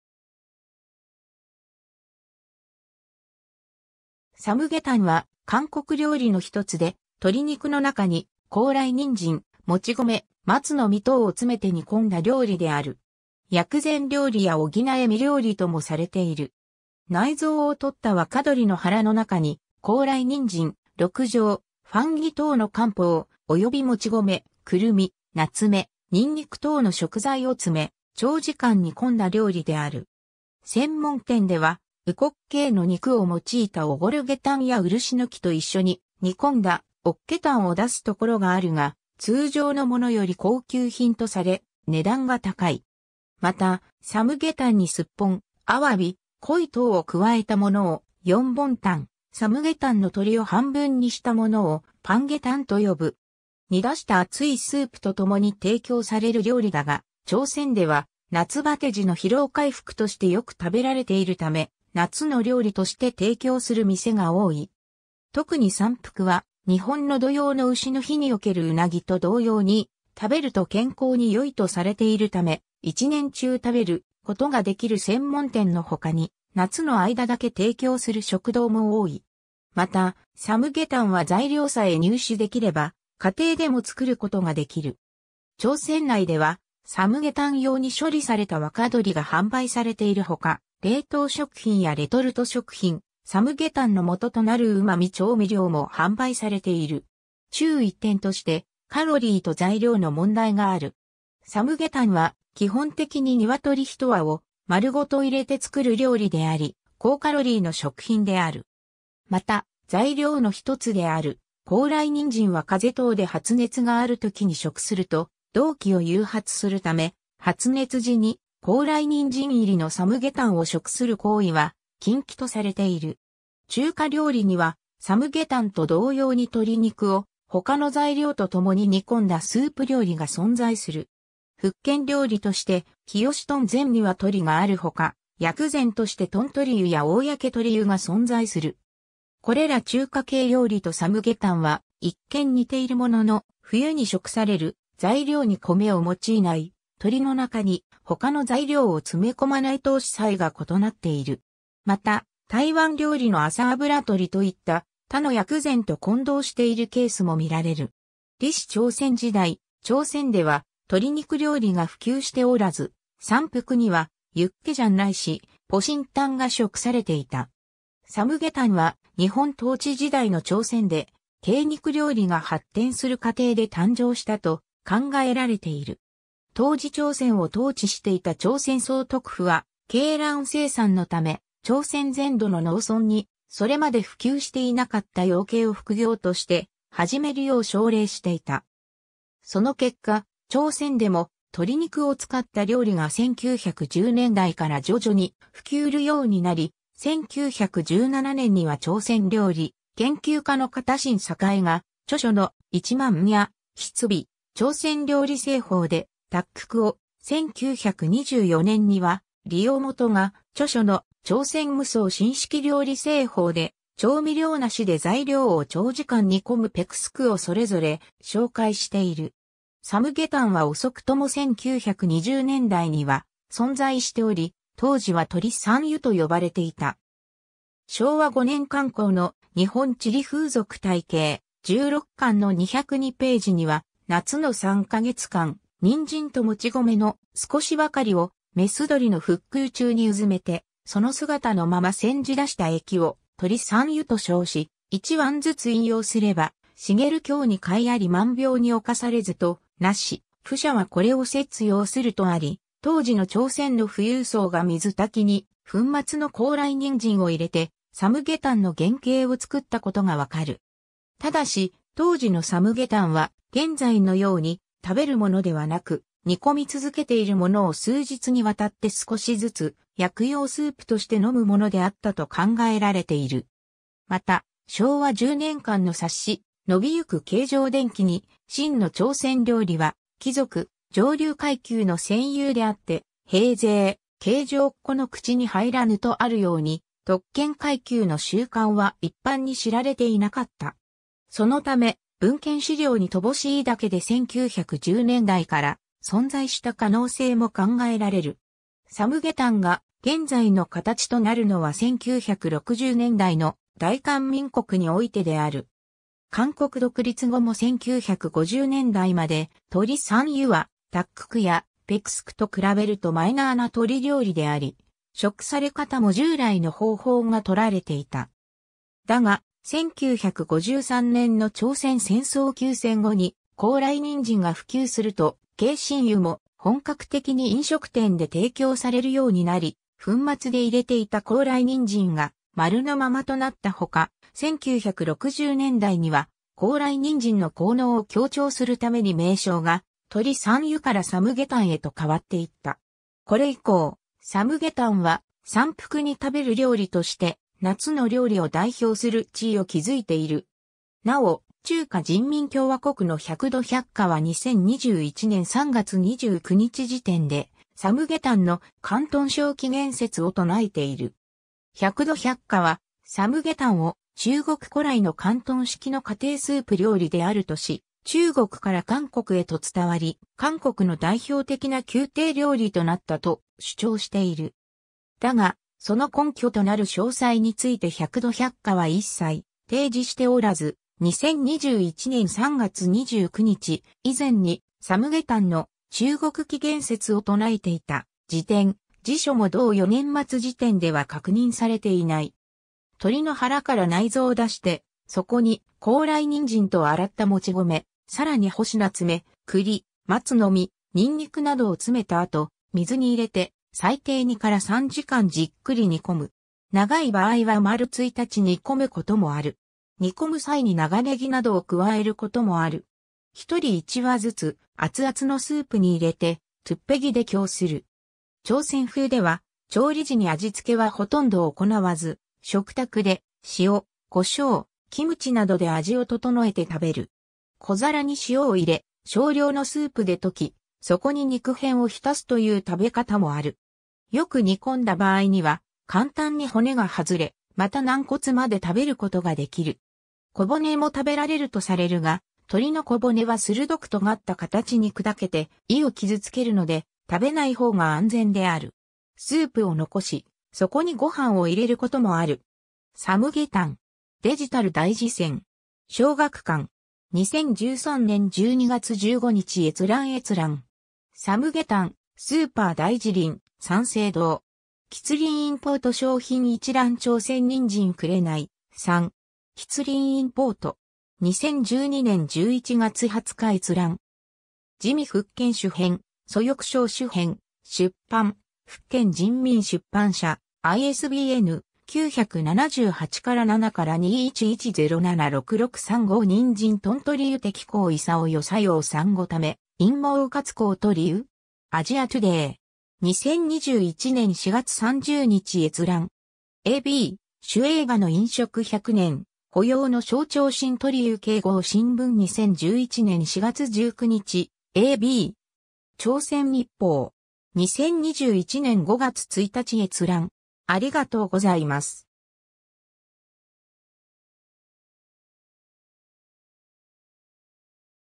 サムゲタン
韓国のスープ・鍋料理
サムゲタン（参鶏湯）は韓国料理の一つで、鶏肉の中に高麗人参、もち米、松の実等を詰めて煮込んだ料理である。
薬膳料理や補身料理（ポシン料理・滋養食）ともされている。
 内臓を取った若鶏（鶏肉）の腹の中に、高麗人参、鹿茸、ファンギ（キバナオウギ）等の漢方、及びもち米、くるみ、ナツメ、ニンニク等の食材を詰め、長時間煮込んだ料理（グク）である。
専門店では、烏骨鶏の肉を用いたオゴルゲタン（烏骨鶏湯、오골계탕）や漆の木と一緒に煮込んだオッケタン（漆鶏湯、옻계탕）を出すところがあるが、通常のものより高級品とされ、値段が高い。
また、参鶏湯にスッポン、アワビ、鯉等を加えたものをヨンボンタン（龍鳳湯、용봉탕）、参鶏湯の鶏を半分にしたものをパンゲタン（半鶏湯、반계탕）と呼ぶ。
 煮出した熱いスープと共に提供される料理だが、朝鮮では夏バテ時の疲労回復としてよく食べられているため、「夏の料理」として提供する店が多い。
特に三伏は日本の土用の丑の日におけるウナギと同様に「食べると健康に良い」とされているため、一年中食べることができる専門店の他に、夏の間だけ提供する食堂も多い。
また、参鶏湯は材料さえ入手できれば家庭でも作ることができる。
朝鮮内ではサムゲタン用に処理された若鶏が販売されている他、冷凍食品やレトルト食品、サムゲタンの素となるうま味調味料も販売されている。
 注意点として、カロリーと材料の問題がある。
参鶏湯は基本的に鶏一羽を丸ごと入れて作る料理であり、高カロリーの食品である（レトルトで一人前とされている800g入りで720kカロリー。大象ジャパンが輸入している製品の場合）。
また、材料の一つである高麗人参は風邪等で発熱がある時に食すると動悸を誘発するため、発熱時に高麗人参入りの参鶏湯を食する行為は禁忌とされている。
 中華料理には、参鶏湯と同様に鶏肉を他の材料と共に煮込んだスープ料理が存在する。
福建料理として清燉全鶏がある他、薬膳として燉鶏湯や公鶏湯が存在する。
これら中華系料理と参鶏湯は一見似ているものの、冬に食される、材料に米を用いない、鶏の中に他の材料を詰め込まない等子細が異なっている。
また、台湾料理の麻油鶏といった他の薬膳と混同しているケースも見られる。
 李氏朝鮮時代、朝鮮では鶏肉料理が普及しておらず、三伏にはユッケジャンないしポシンタンが食されていた。
 参鶏湯は、日本統治時代の朝鮮で鶏肉料理が発展する過程で誕生したと考えられている。
当時朝鮮を統治していた朝鮮総督府は鶏卵生産のため、朝鮮全土の農村にそれまで普及していなかった養鶏を副業として始めるよう奨励していた。
その結果、朝鮮でも鶏肉を使った料理が1910年代から徐々に普及るようになり、1917年には朝鮮料理研究家の方信榮が著書の『萬家必備・朝鮮料理製法』（京城・新文館発行）でタックク（닭국、鶏肉の入ったグク）を、1924年には李用基が著書の『朝鮮無双新式料理製法』（京城・永昌書館発行）で調味料無しで材料を長時間煮込むペクスク（백숙、丸鶏の水炊き）をそれぞれ紹介している。
 参鶏湯は遅くとも1920年代には存在しており、当時は鶏参湯（ケサムタン、けいじんとう）と呼ばれていた。
昭和5年（1930年）刊行の『日本地理風俗大系・十六巻』（新光社）の202頁には 「夏の三カ月間、人参と糯米の少しばかりを雌鶏の腹腔中にうずめて、その姿のまま煎じ出した液を鶏参湯（けいじんとう）と称し、一碗ずつ飲用すれば、滋強に効あり万病に冒されずとなし、富者はこれを摂用する」とあり、当時の朝鮮の富裕層が水炊きに粉末の高麗人参を入れて参鶏湯の原型をつくったことが分る。
ただし、当時の参鶏湯は現在のように「食べる」ものではなく、煮込み続けているものを数日にわたって少しずつ薬用スープとして飲むものであったと考えられている。
また、昭和10年（1935年）刊の冊子『伸びゆく京城電気』に「真の朝鮮料理は貴族・上流階級の占有であって、平生、京城っ子の口に入らぬ」とあるように、特権階級の習慣は一般に知られていなかった。
そのため、文献資料に乏しいだけで1910年代から存在した可能性も考えられる。
 参鶏湯が現在の形となるのは1960年代の大韓民国においてである。
韓国独立後も1950年代まで鶏参湯はタッククやペクスクと比べるとマイナーな鶏料理であり、食され方も従来の方法が採られていた。
だが、1953年の朝鮮戦争休戦後に高麗人参が普及すると鷄蔘湯も本格的に飲食店で提供されるようになり、粉末で入れていた高...